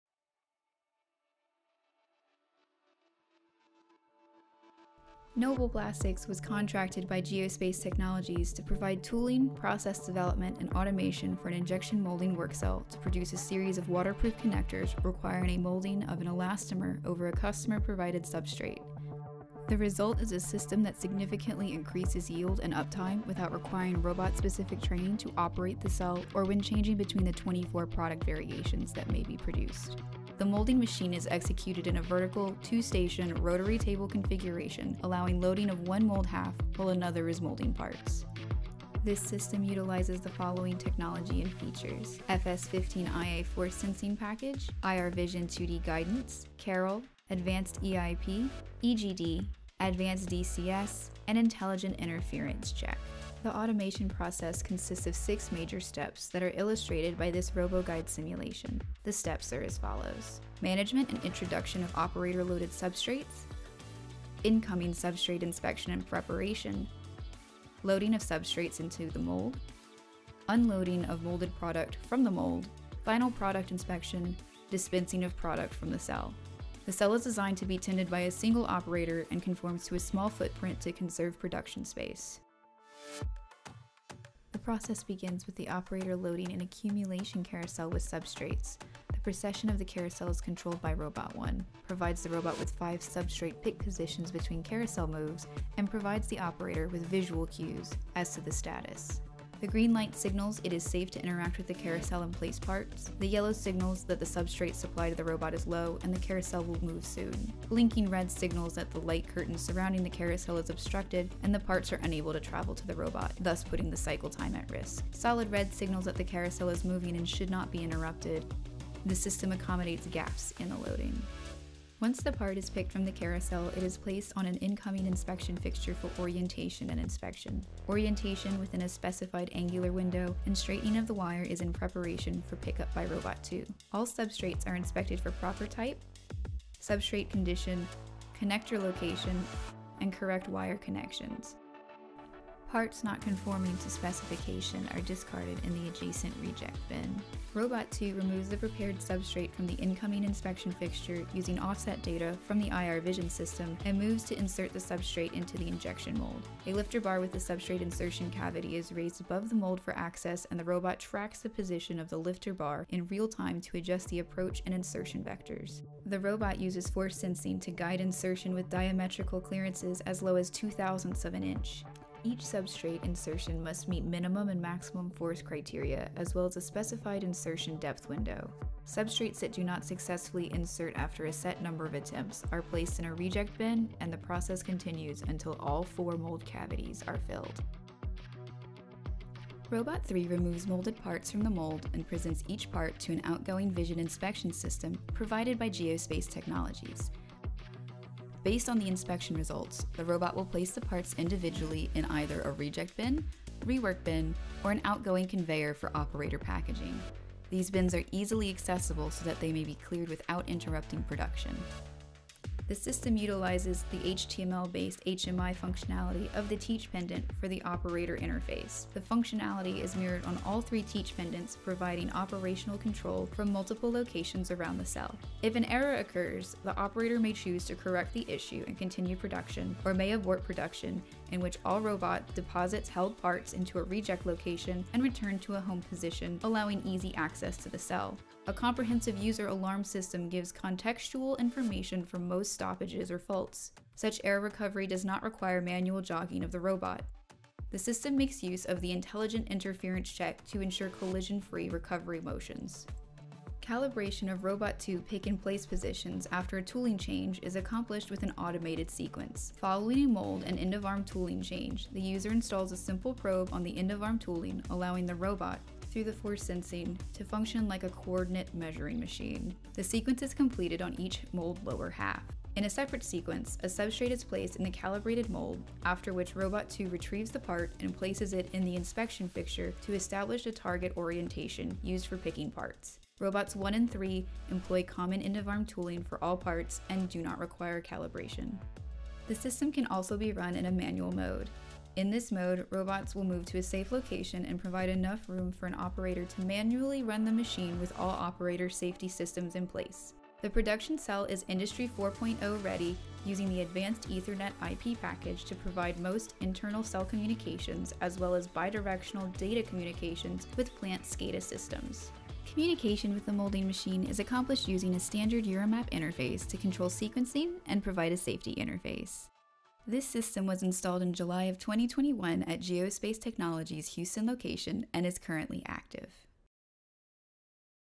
Automation for Geospace Technologies: FANUC America System of the Year 2022 Finalist
Contracted by Geospace Technologies, Noble Plastics created a custom automation cell to assist in the management, inspection, and over-molding of a customer provided substrate. Submitted and accepted as a finalist for FANUC America's System of the Year 2022, join us as we break down the technology we used to provide the customer with an efficient and easy to use cell.